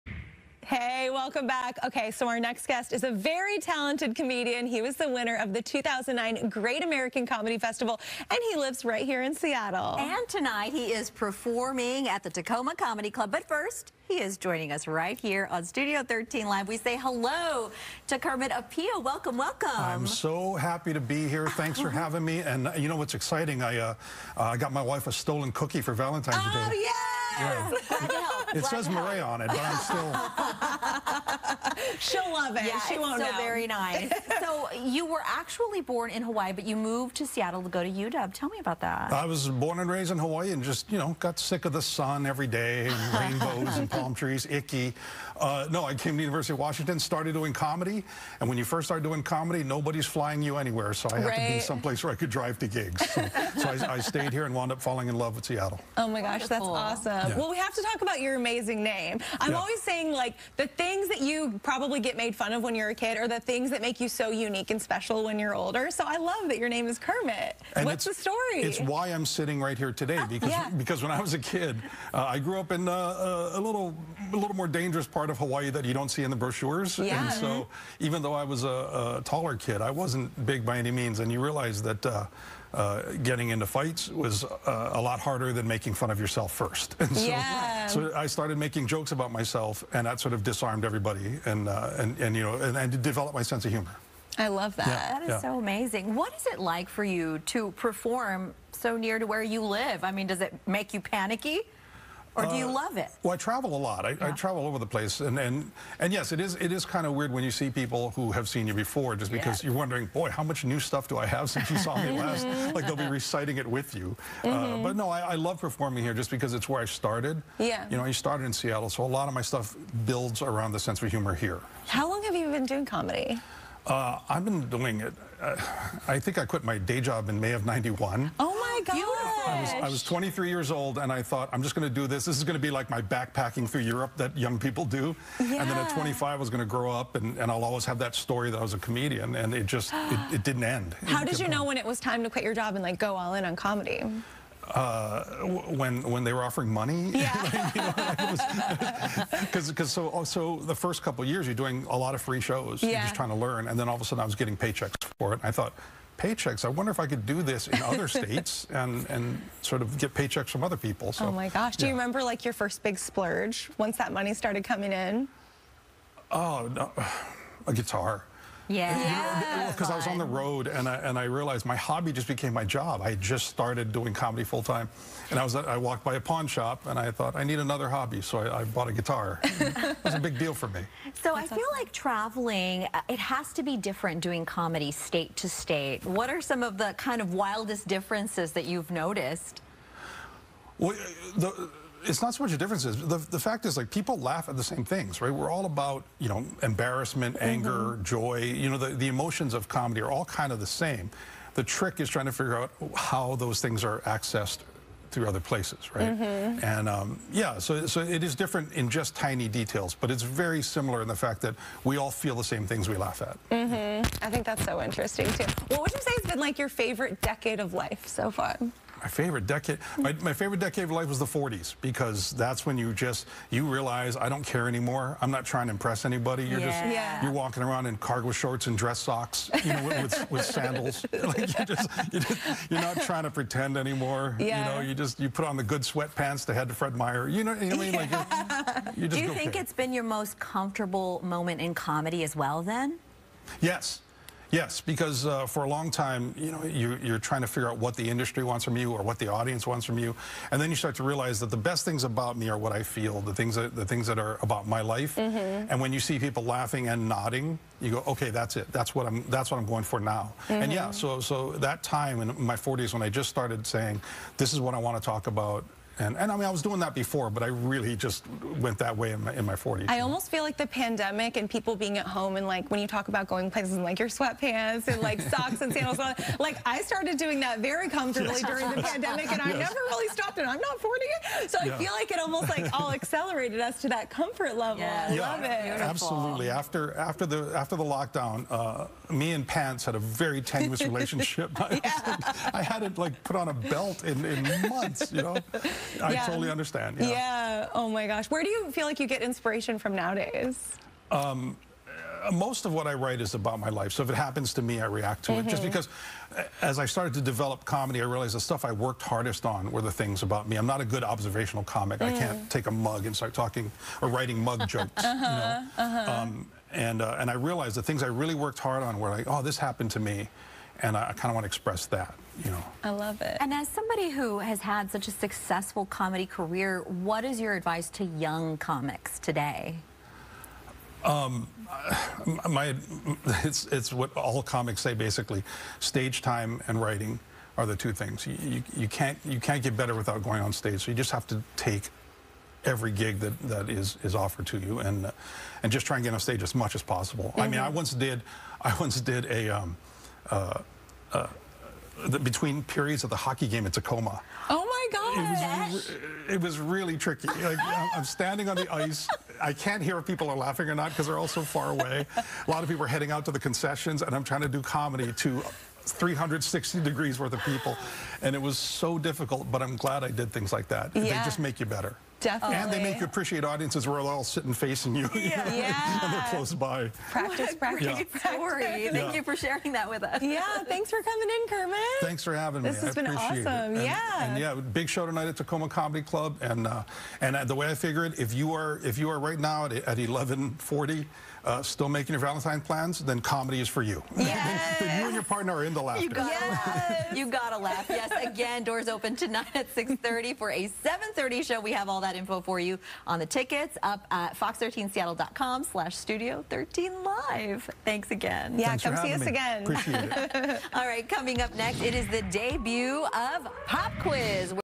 Comedian Kermet Apio discusses career ahead of show at Tacoma Comedy Club
While he was born in Hawaii, comedian Kermet Apio moved to Seattle to attend the University of Washington. In 1989, Apio did his first open mike night at the Comedy Underground in Seattle. He's made Seattle home and is performing at the Tacoma Comedy Club on Valentine's Day.